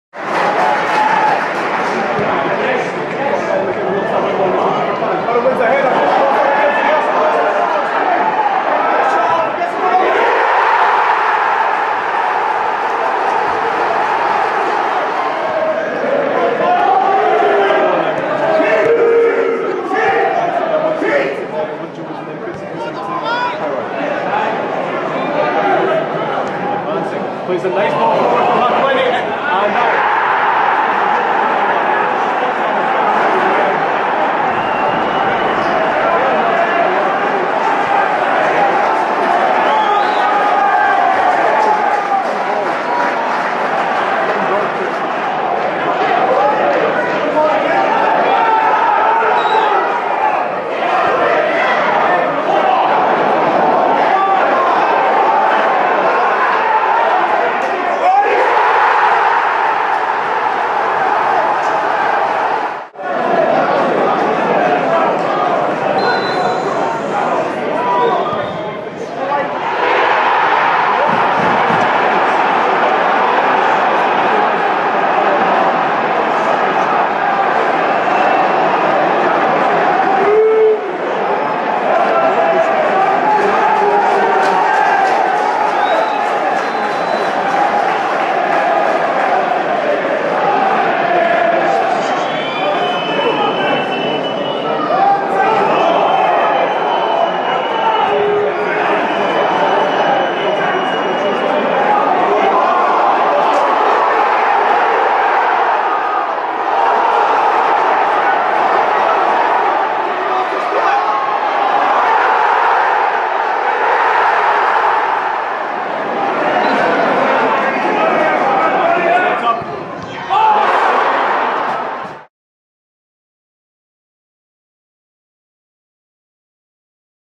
Just The Goals | Dundee 2-2 Kilmarnock
Conor McAleny scored on his Killie debut as Dan Bachmann saved a last-gasp penalty